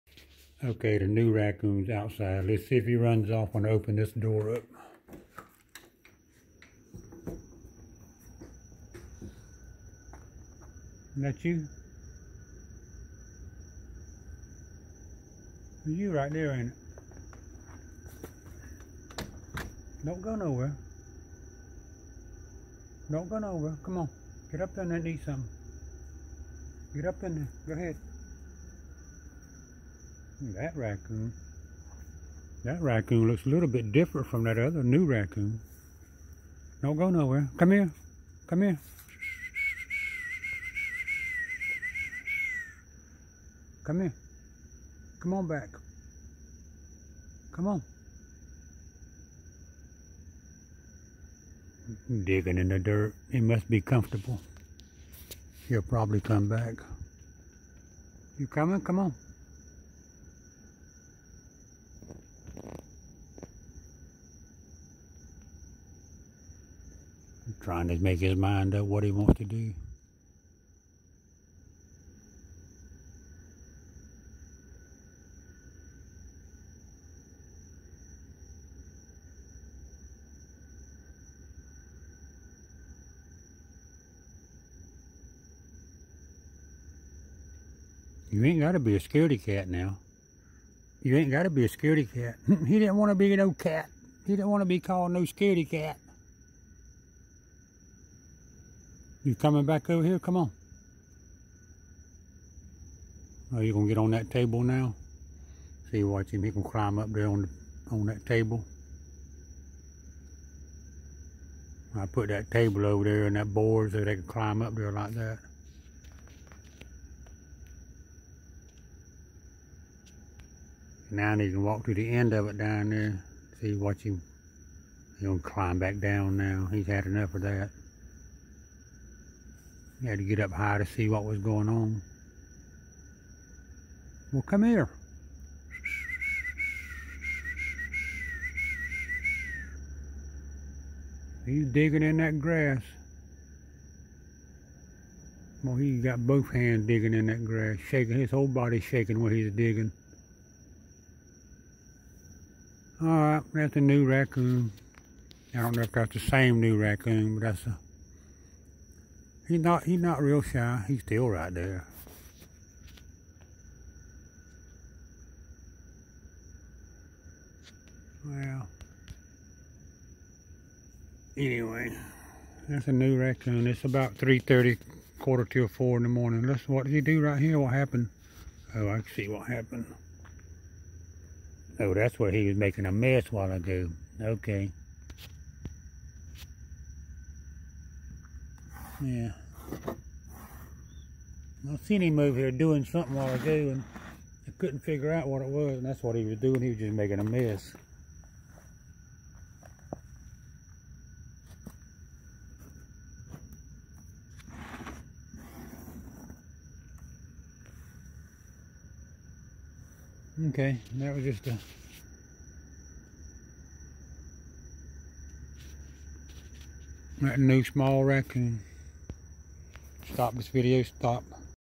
New Small Raccoon.. Just Messing Around
He is just messing around and not worry about me.. Just being lazy.. he is getting on the picnic table and looking around At his new home.. I hope..